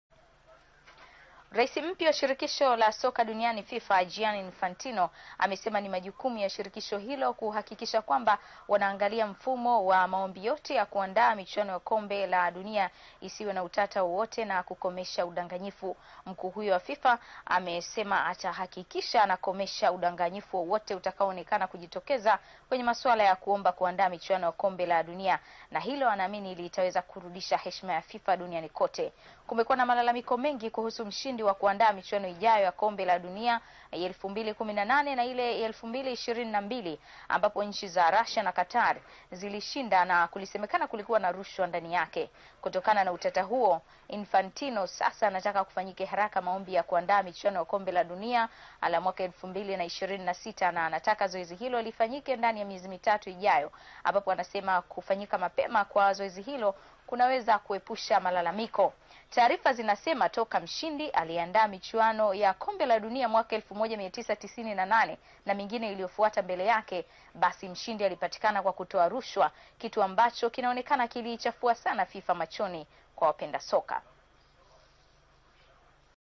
Rais Wa FIFA Gianni Infantino Aahidi Kuondoa Rushwa Kombe La Dunia
Rais mpya wa FIFA Gianni Infantino amesema atahakikisha wanaangalia mfumo wa maombi yote ya kuandaa michunao ya kombe la dunia.